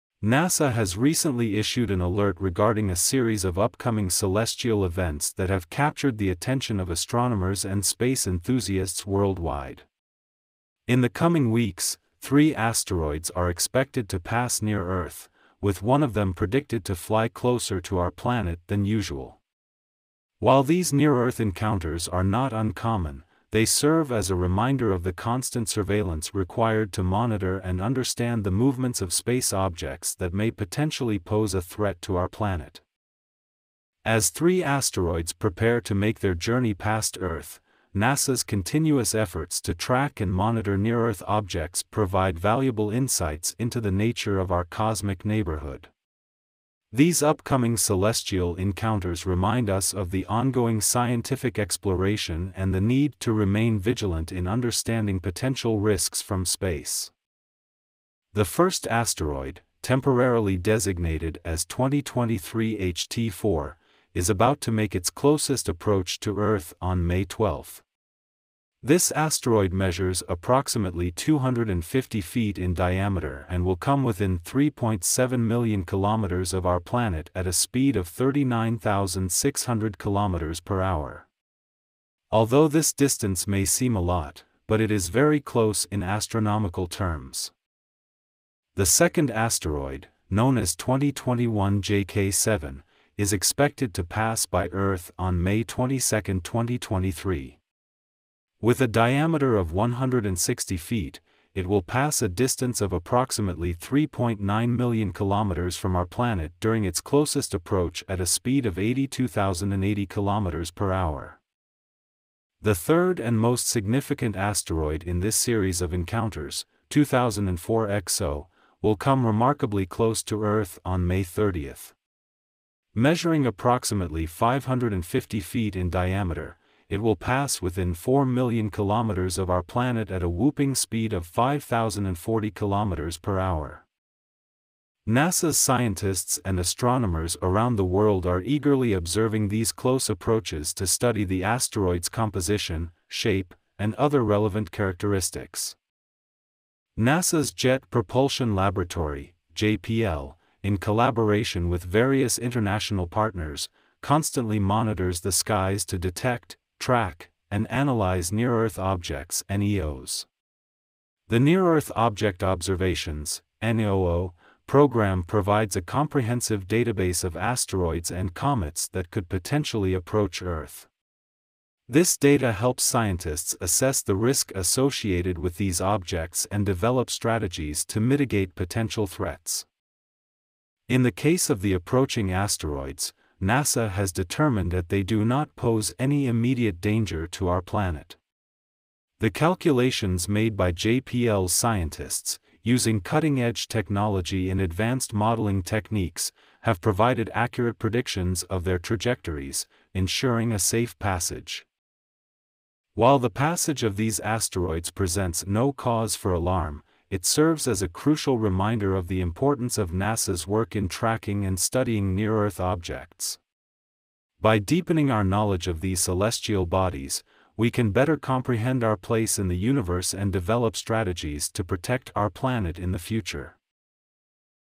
NASA raises warning of three massive asteroids heading towards Earth in May 2023
NASA has issued an alert regarding three asteroids expected to pass near Earth in the coming weeks. One asteroid will come closer to Earth than usual, but NASA has determined that none of them pose an immediate danger to our planet. NASA's continuous efforts to track and monitor near-Earth objects provide valuable insights into the nature of our cosmic neighborhood, and the upcoming celestial encounters serve as a reminder of the need to remain vigilant in understanding potential risks from space. NASA's Jet Propulsion Laboratory constantly monitors the skies to detect, track, and analyze near-Earth objects, and the data gathered helps scientists assess risks and develop strategies to mitigate potential threats.